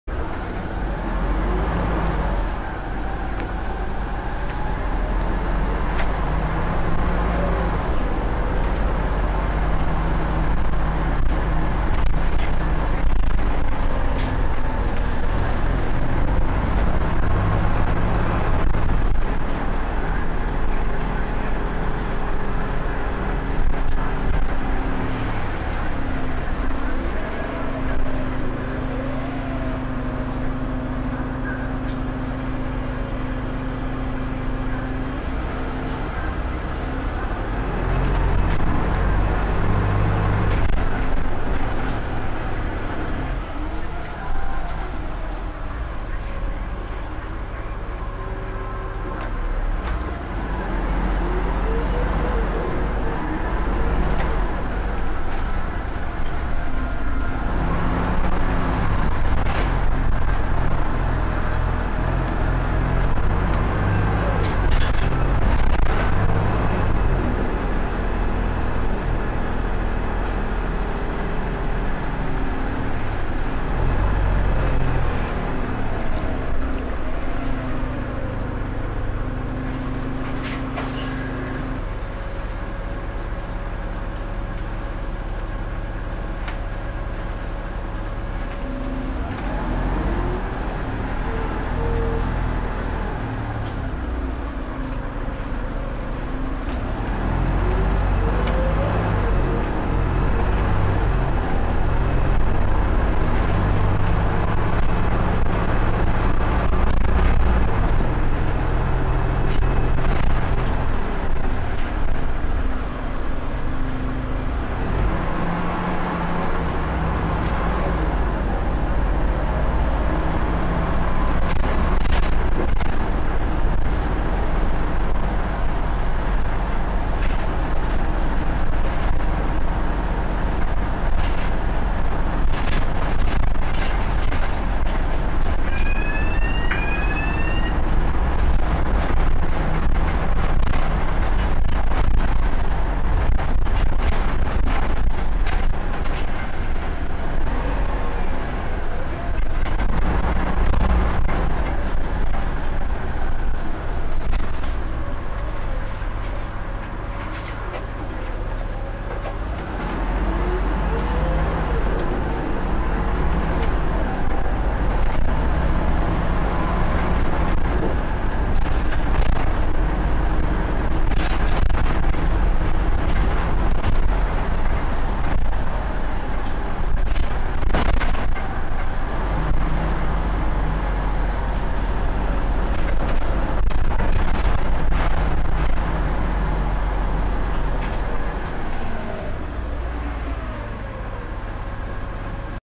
Transdev Burnley Volvo B7RLE Eclipse 2 1864 Route M3
Last day of the old Mainline buses